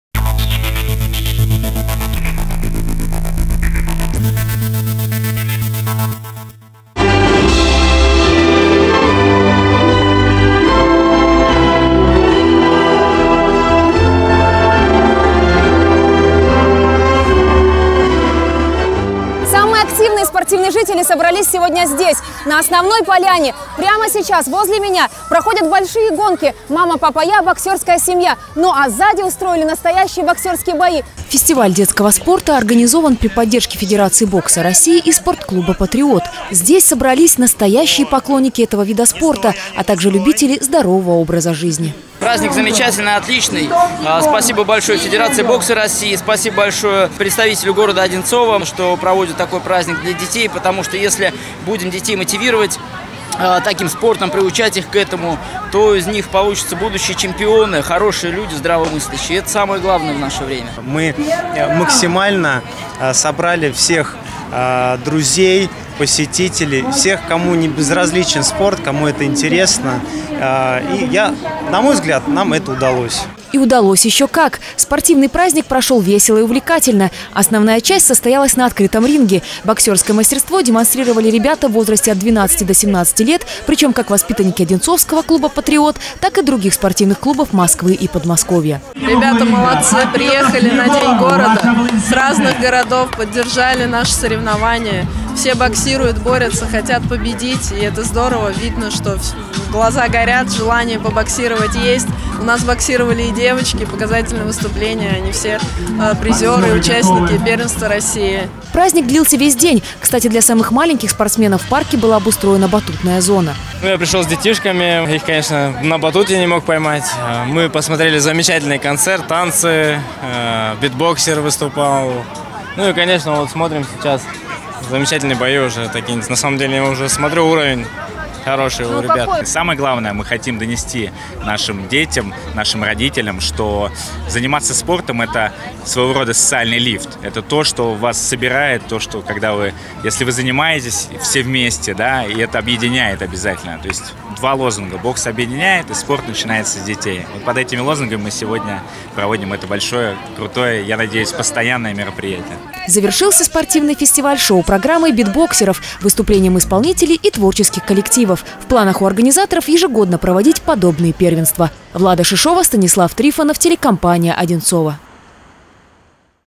Первый фестиваль детского спорта
В день рождения г. Одинцово в Парке культуры, спорта и отдыха имени Ларисы Лазутиной прошел первый фестиваль детского спорта.